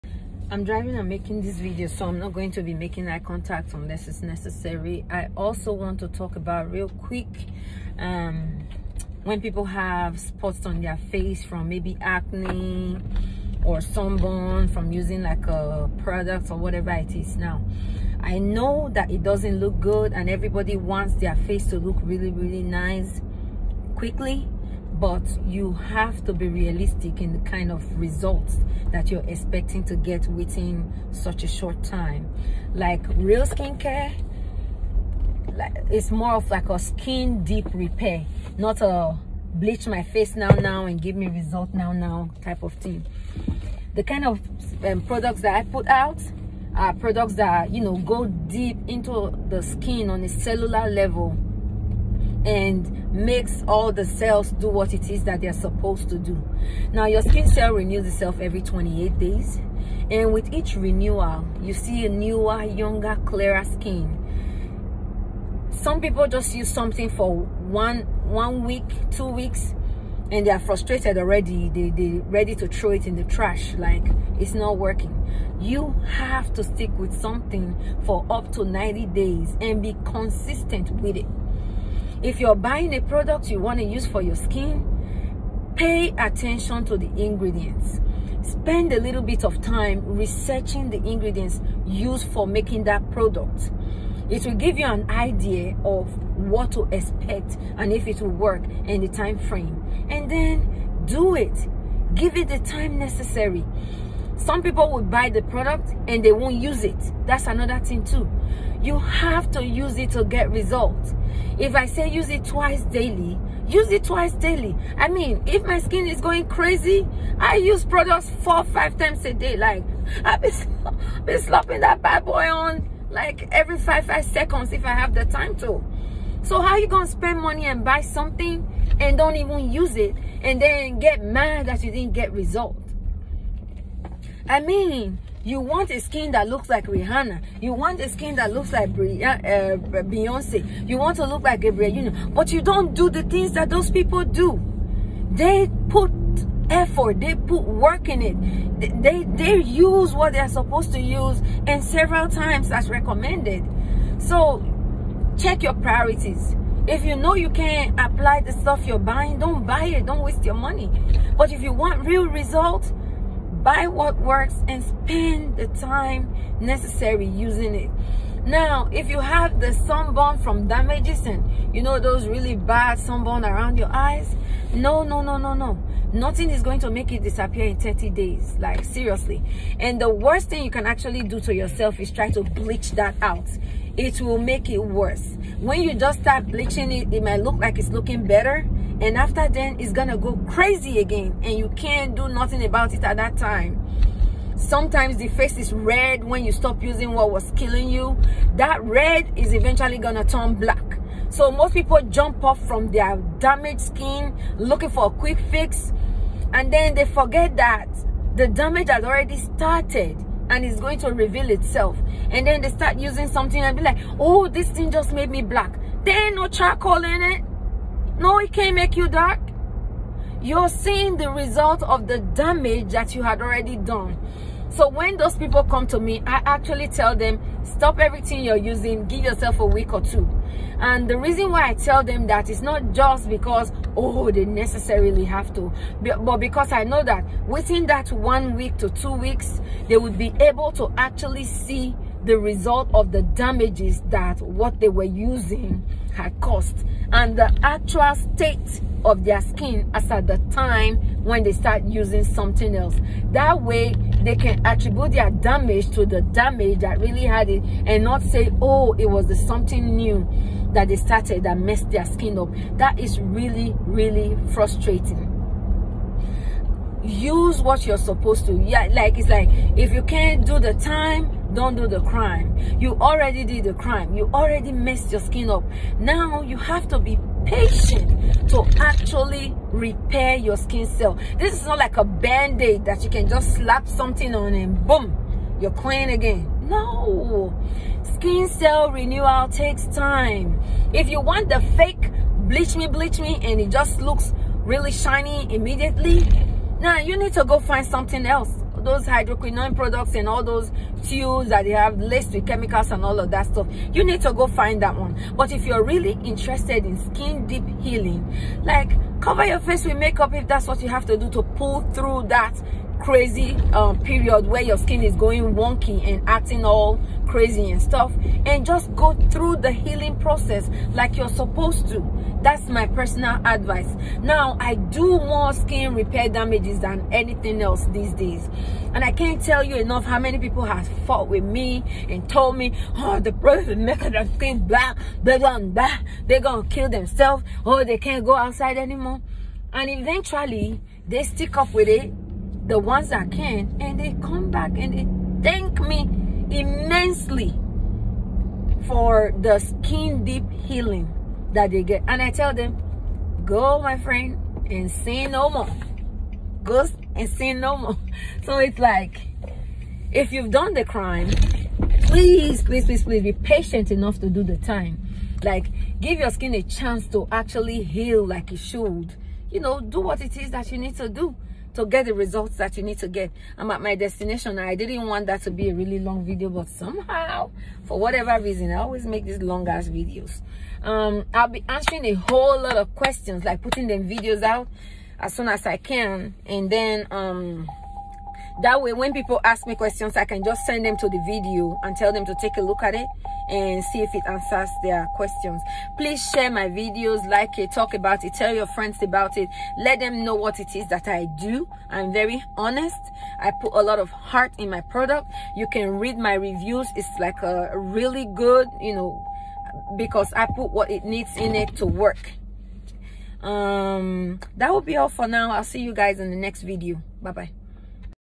Acne spots hyperpigmentation. Goldnskin
My opinion on getting rid of acne spots will make a follow up video on what we use to get rid of acne scars and  sunburn.